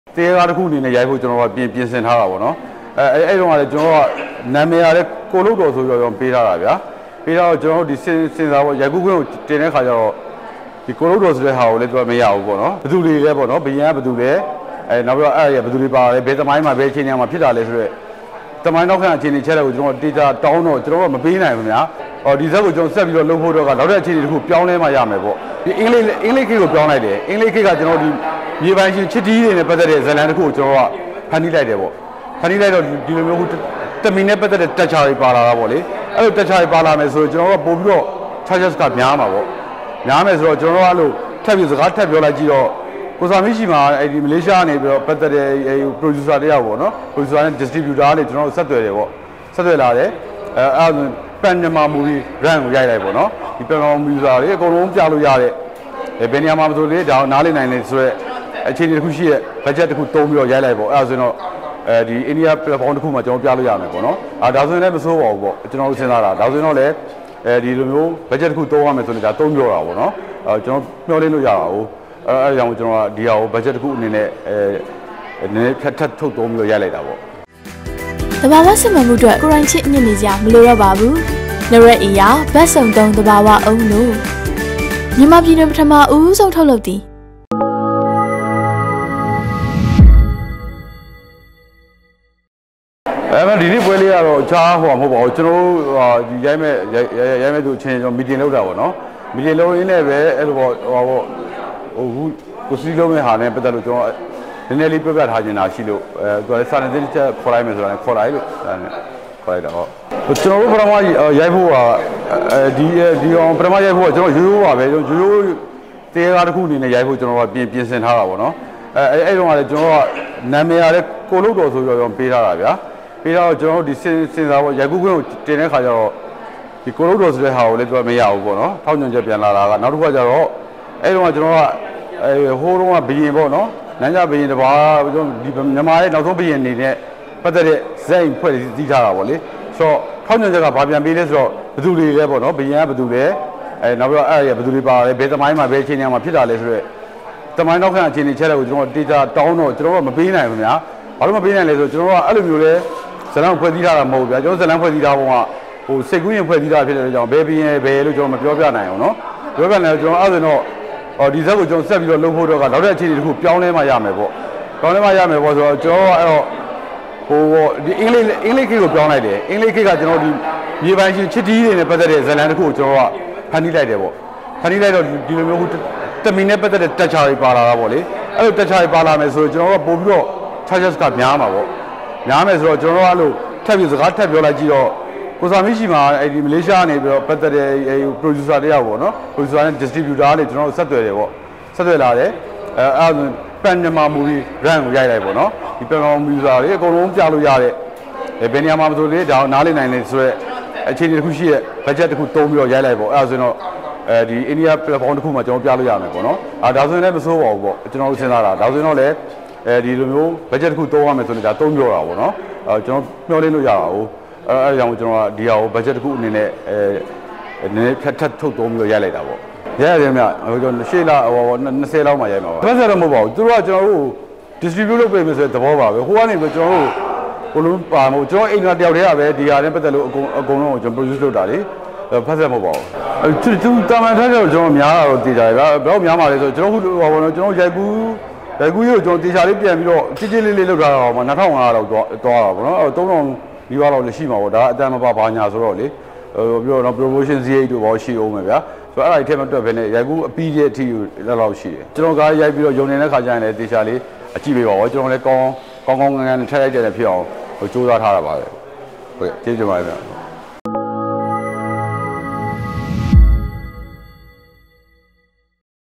ကိုယ်လုပ်တော် နာမည်ကို ဆင်ဆာက ခွင့်မပြုခဲ့လို့ နာမည်ပြောင်းပေးခဲ့ရတယ် - ကိုအောင်ကြီး
ကိုယ်လုပ်တော် ကနေ မင်းကောင်းမင်းမြတ် လို့ ပြောင်းခဲ့ပေမယ့် ဆင်ဆာက ခွင့်မပြုလို့ မောင်မင်းကောင်းနဲ့ မောင်မင်းမြတ် ဖြစ်သွားခဲ့ရတဲ့ဇာတ်ကား - ကိုအောင်ကြီး(ထုတ်လုပ်သူ၊ Indra Film Production)
"ဒီဇာတ်ကားကို ရိုးရိုးသာမန် သရဲကားတစ်ကားအနေနဲ့ ရိုက်ကူးဖို့ ပြင်ဆင်ထားခဲ့တာ။ ဇာတ်ကားနာမည်ကိုလည်း ကျွန်တော်တို့က ကိုယ်လုပ်တော် လို့ ပေးထားတာ။ ဆင်ဆာရိုက်ကူးခွင့်ကို တင်တဲ့အခါကျတော့ ကိုယ်လုပ်တော် ဆိုတဲ့ နာမည်က မရဘူးလို့ ထောက်ချက်ပြန်လာတယ်ပေါ့။ အဲ့ဒီတုန်းက ဟိုတုန်းကဘုရင်တစ်ပါး မြန်မာရဲ့နောက်ဆုံးဘုရင်နဲ့ပတ်သက်တဲ့ ဇာတ်အိမ်ဖွဲ့ထားတာပေါ့နော်။ ဆင်ဆာဘက်က ဘုရင်က ဘယ်သူလဲ၊ ဘယ်သူတွေ ပါမှာလဲ၊ ဘယ်သမိုင်း ဘယ်အချိန် ဘယ်နေရာမှာဖြစ်တာလဲဆိုတဲ့ သမိုင်းနောက်ခံအချက်အလက်တွေကိုတောင်းတော့ ကျွန်တော်တို့က မပေးနိုင်ဘူးဗျာ။ ဘာလို့ မပေးနိုင်တာလဲဆိုတော့ ကျွန်တော်တို့က ဇာတ်လမ်းကို သမိုင်းကြီးနဲ့ အဲ့ဒီလိုကြီး ဖွဲ့စည်းထားတာ မဟုတ်ဘူးလေ။ ဇာတ်လမ်းဖွဲ့စည်းထားတာက စိတ်ကူးယဉ်ဖွဲ့စည်းထားတာလေ။ အဲ့ဒါနဲ့ ဒီဇာတ်ကို လုပ်ဖို့အတွက်က ကျွန်တော်တို့ နောက်ထပ်အခြေအနေတစ်ခုကို ပြောင်းမှ ရမယ်ဆိုပြီး အင်္ဂလိပ်ခေတ်ကို ပြောင်းလိုက်တယ်။ မြေပိုင်ရှင် ချစ်တီးတွေနဲ့ပတ်သက်တဲ့ဇာတ်လမ်းတစ်ခုကို ကျွန်တော်တို့ ပြောင်းလဲဖန်တီးလိုက်ရတယ်" - ကိုအောင်ကြီး(ထုတ်လုပ်သူ၊ Indra Film Production)